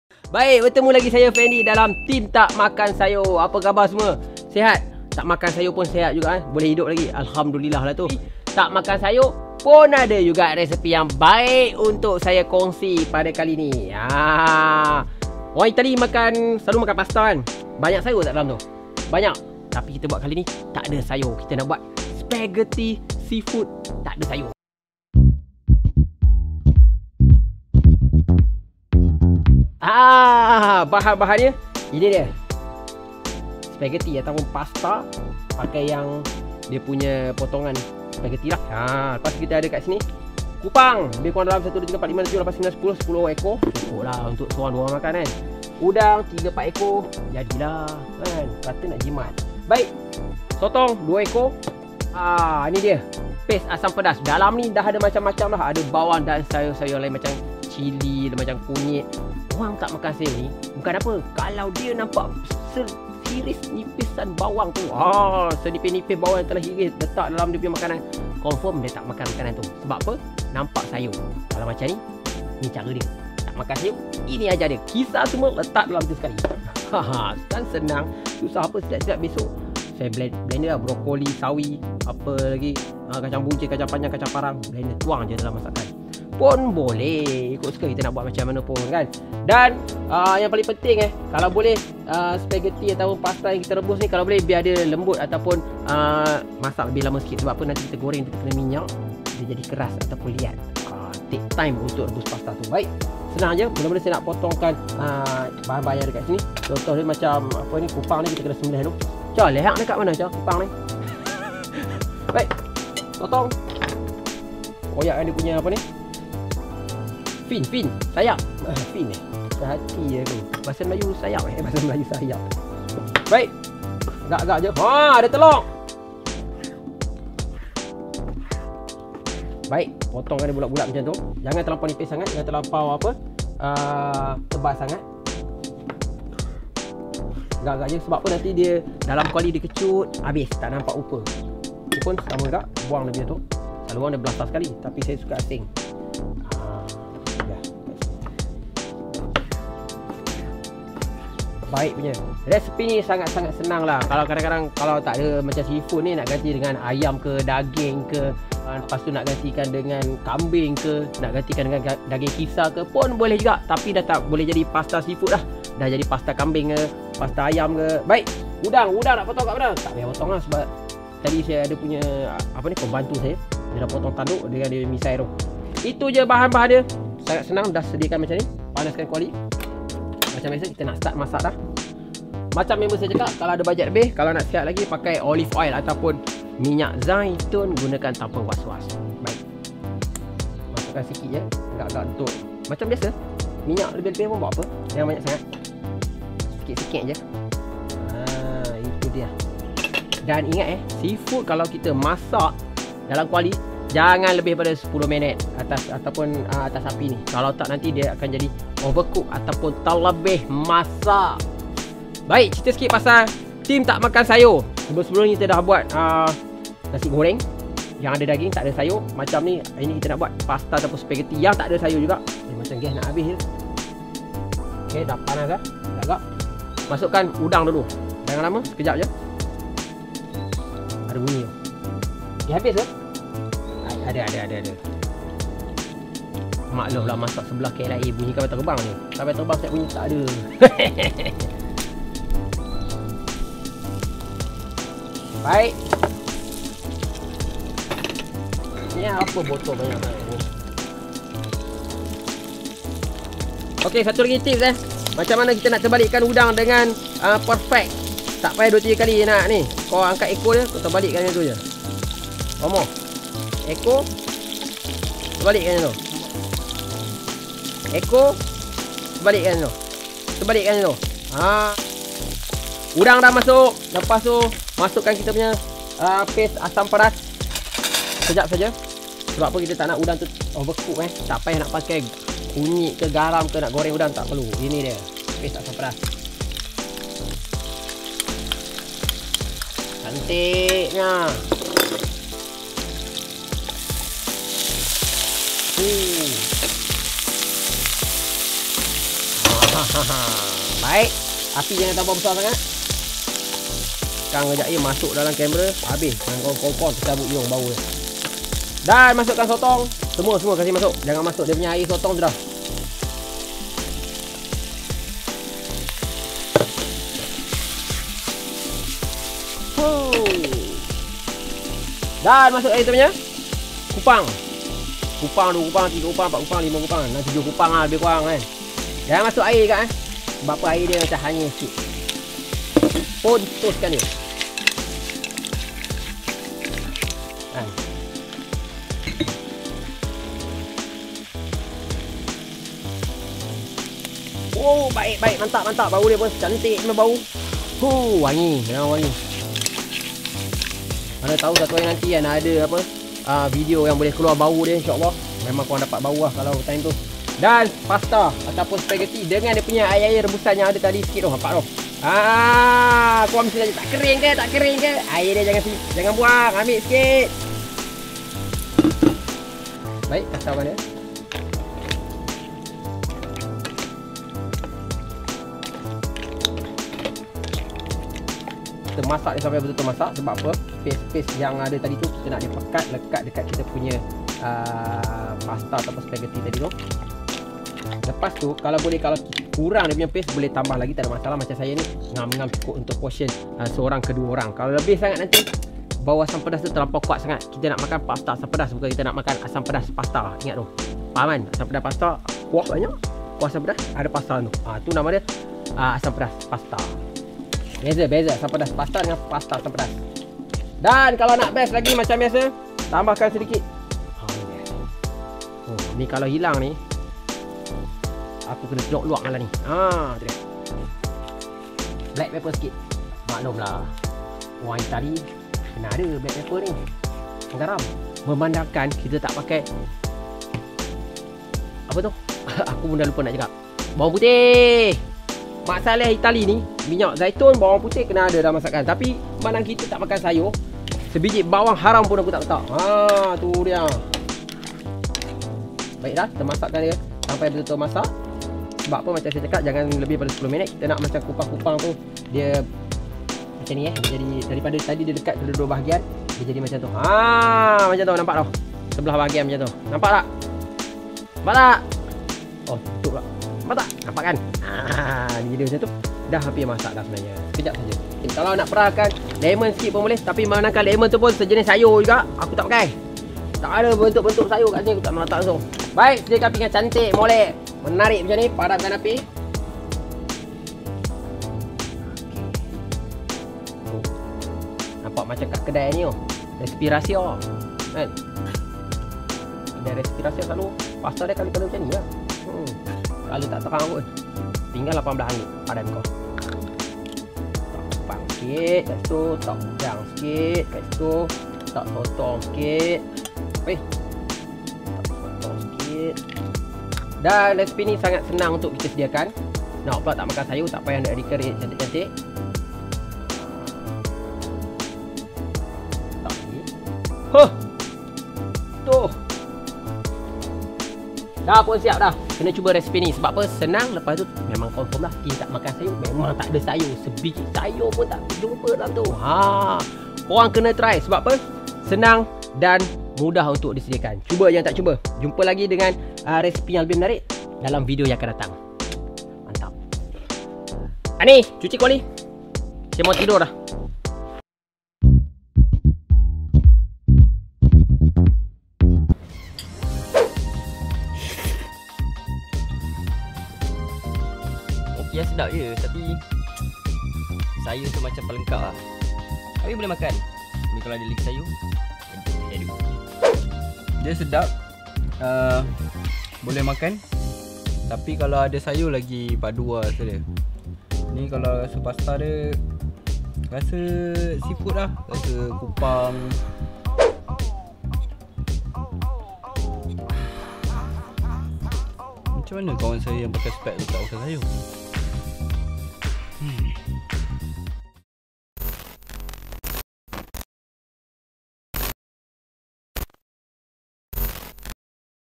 TeamTakMakanSayur: Spagetthi Seafood Takde Sayur.
Zaman sekarang ni, makin ramai kawan-kawan kita yang tak makan sayur. Pergi kedai makan mana-mana, nak tunggu dia alih sayur dalam makanan tu lama gila. Kadang kesian pun ada.

Disebabkan kesian dengan orang-orang yang degil macam ni, Fendi Muhammad bagilah resepi spagetthi seafood tak nak sayur.

Tag sekali kawan korang yang tak suka makan sayur tu. Tak pun tag mak korang supaya mak korang boleh masakkan nasi goreng takde sayur untuk korang!